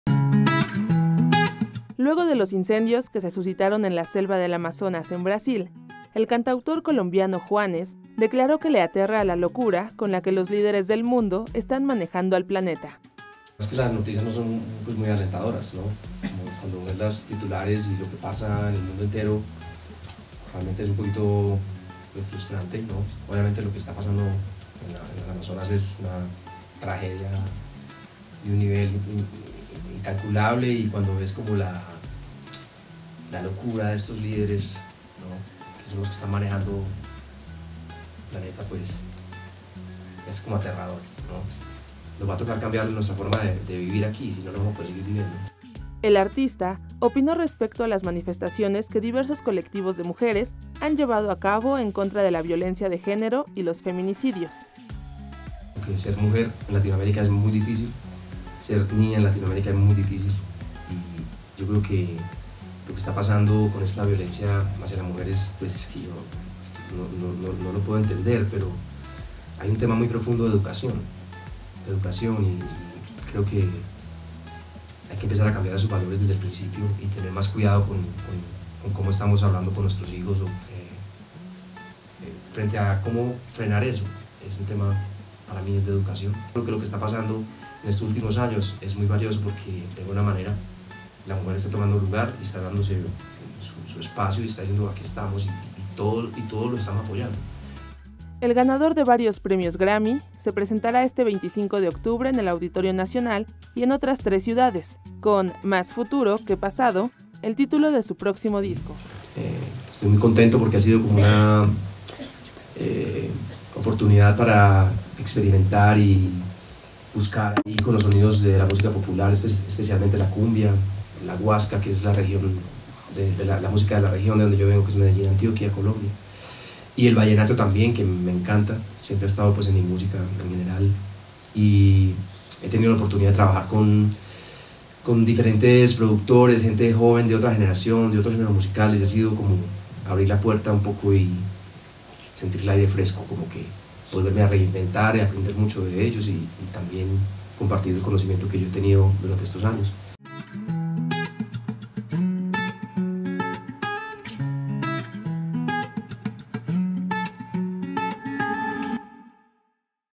"Es aterradora la locura de los líderes que manejan el planeta": Juanes
El cantautor colombiano opina que la violencia se frena con educación.